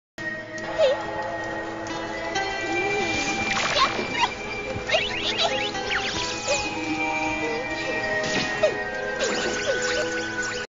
Spiky eared Pichu
She's so beautiful, her voice by Kayzie Rogers.